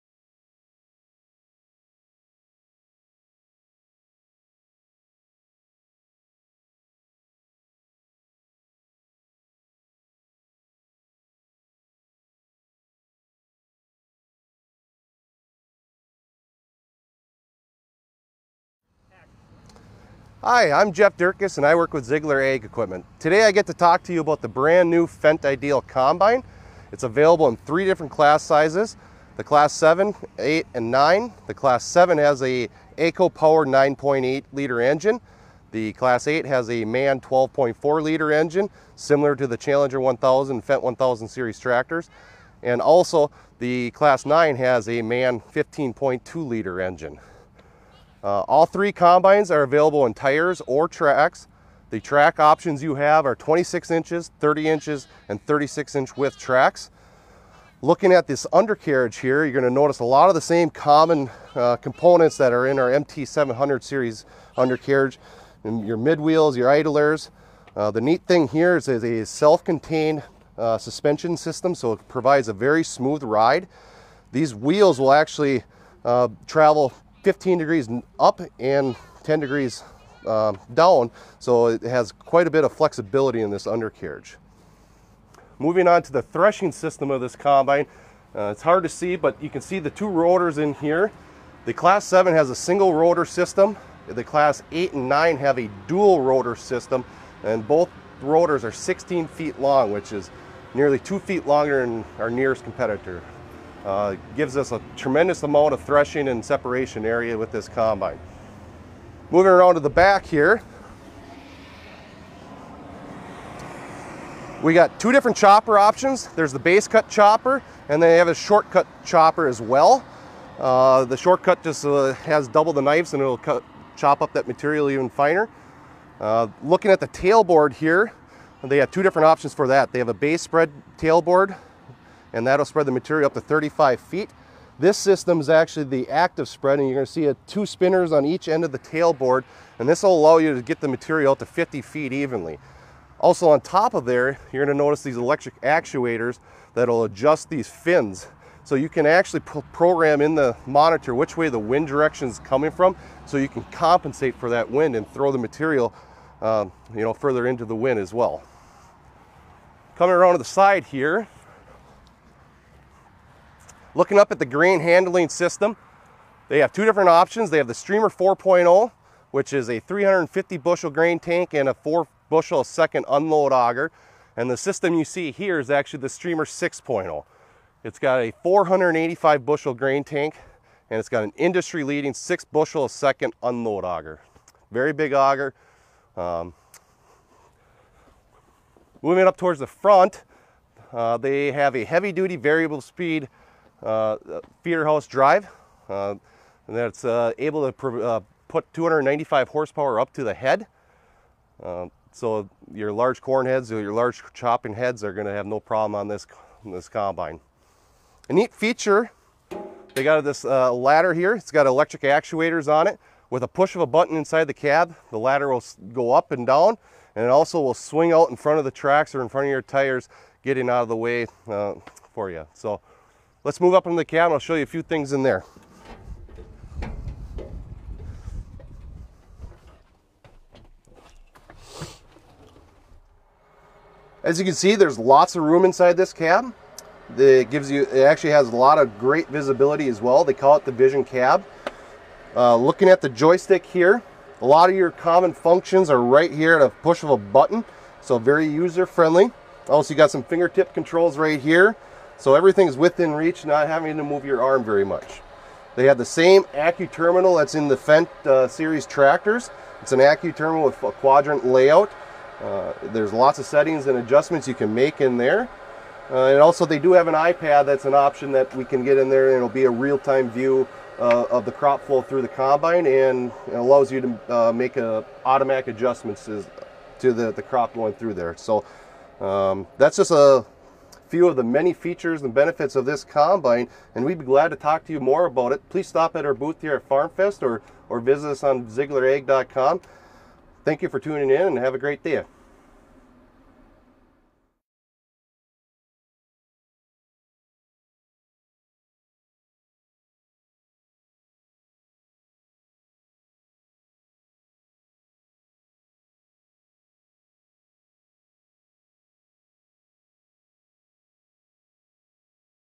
IDEAL COMBINE - Ziegler Ag Equipment
Fendt’s most innovative combine is here at Ziegler. The new IDEAL™ combine from Fendt is engineered to put you in total control of your operation.

The IDEAL combine is designed with the largest grain tank in its class and a fully-automated processing system, saving you extra time and effort during the busy harvest season. Its advanced threshing capabilities provide you with a more efficient output in any condition. A simple cleaning system makes for easy operation, while high-tech sensors bring more intelligence to your harvest. For the cutting edge in performance and innovation, experience the Fendt® IDEAL combine.

Jeff Dirkes walks us through the Fendt IDEAL Combine at Farm Fest 2019.